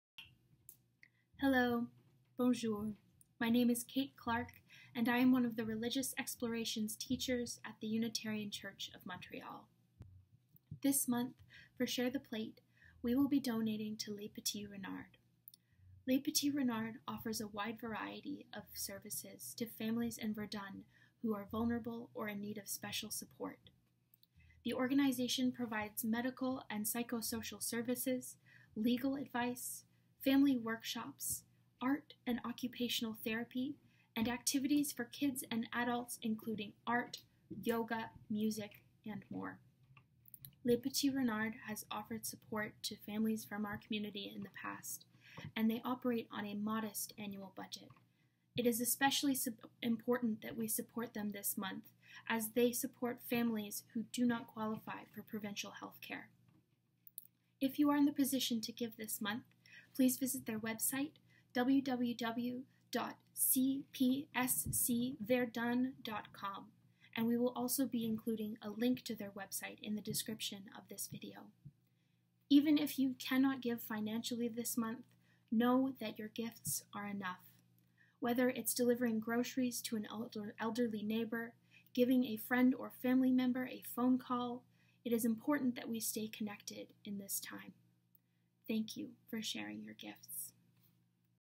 April Share the Plate UCM with Caite Clark
This month, please consider giving a gift to Les petits renards centre de pédiatrie sociale/social pediatrics centre in Verdun. Les petits renards provides support and care for infants and children up to the age of 14, including for families who are not in the Quebec health care system. You can make a direct contribution through their